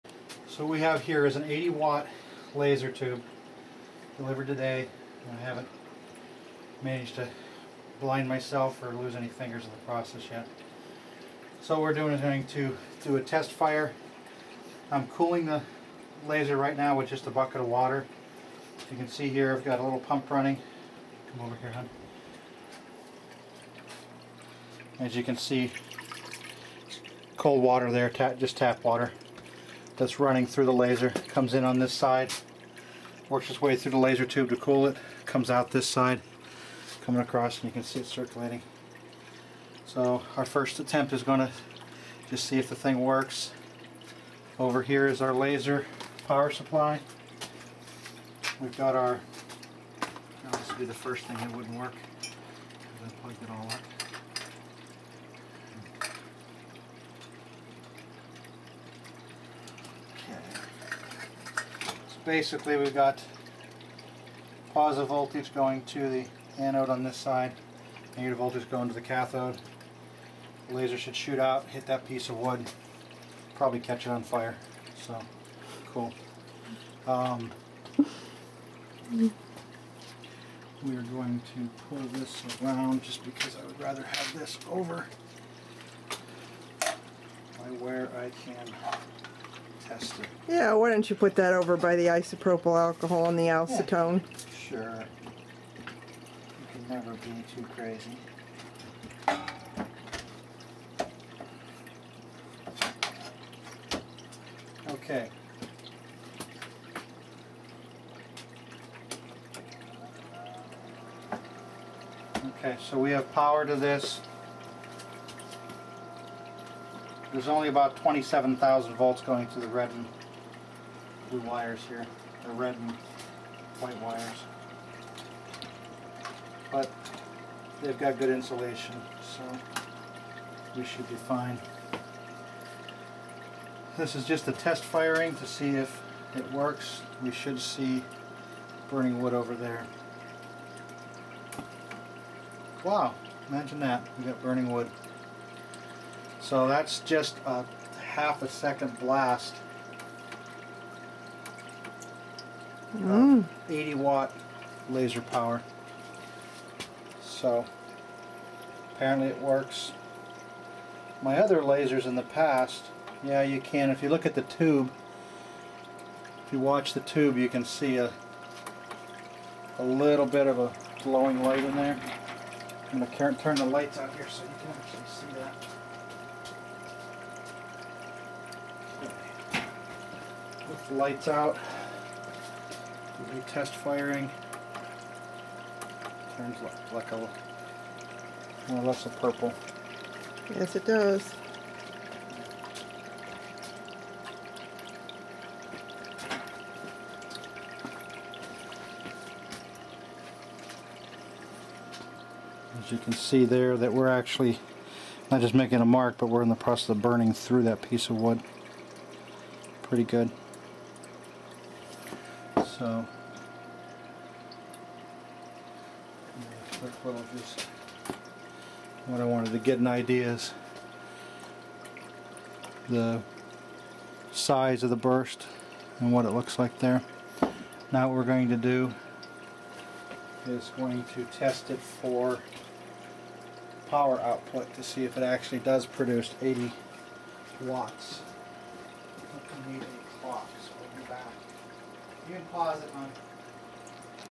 Testing my new 80W RECI Laser Tube
First firing of me new RECI 80W Laser tube. I was able to confirm the output was 85W while the current was kept at about 22mA at 100% power.